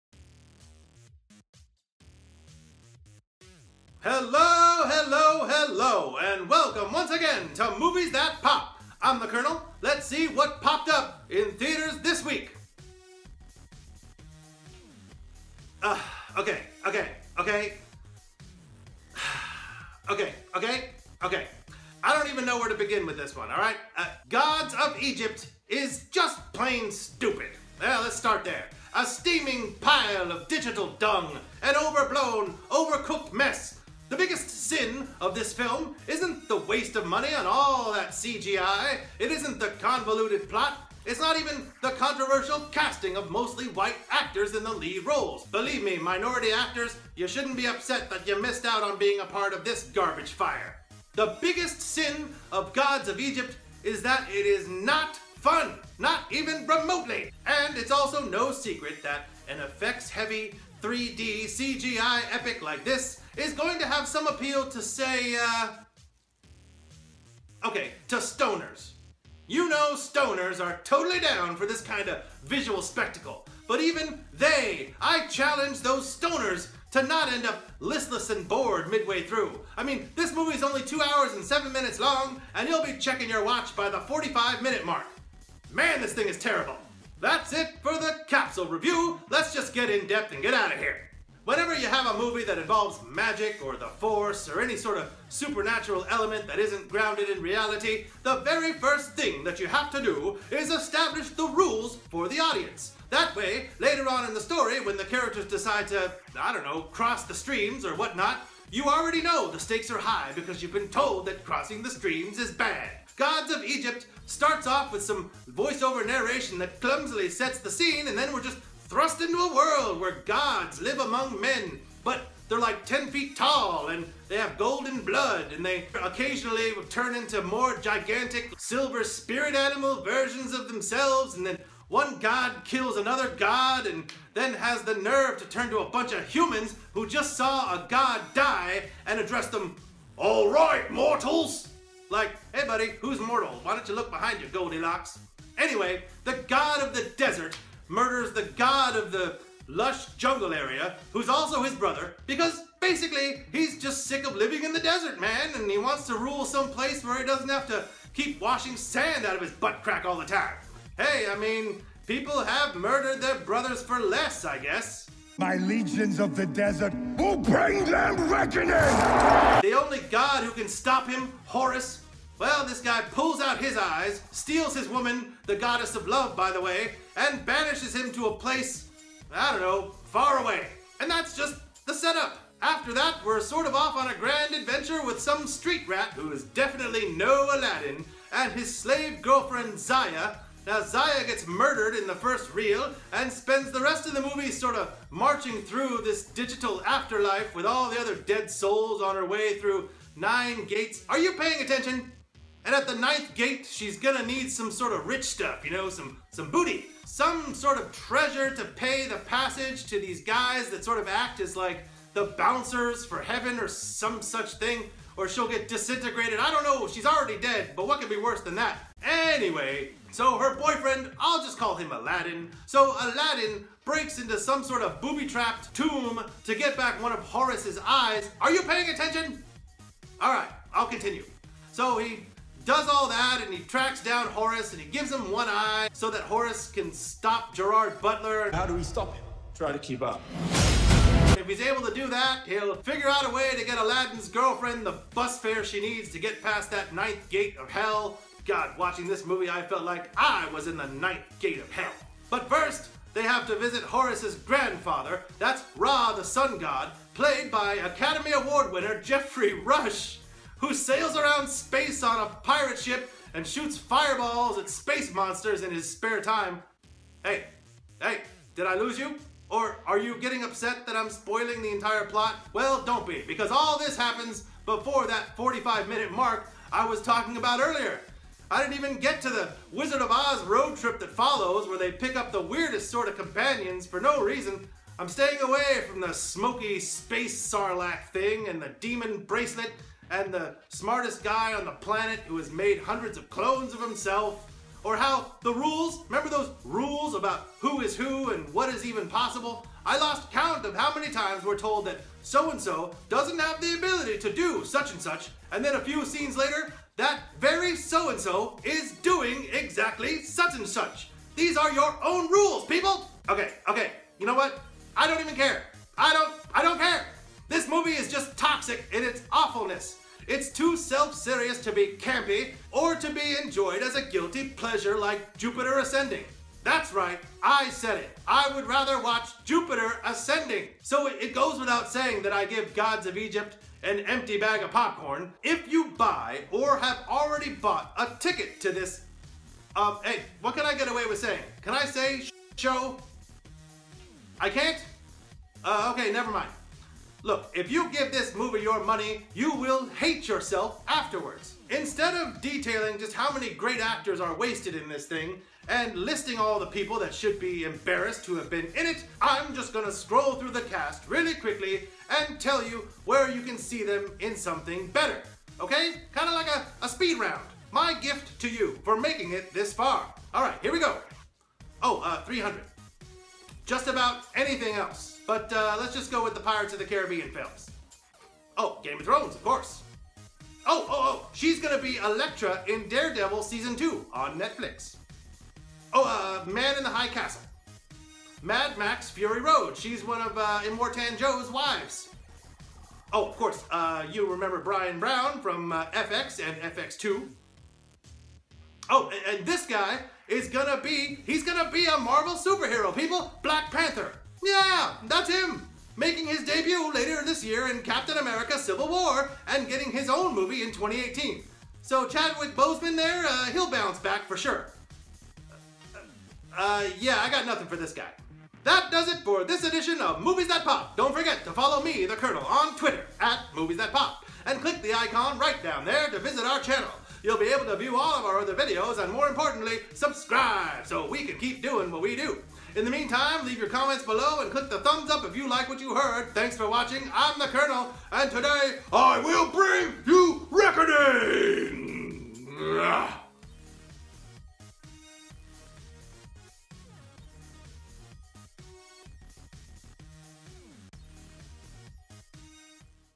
Gods of Egypt - Movie Review
The Kernel reviews the film "Gods of Egypt" in his movie review.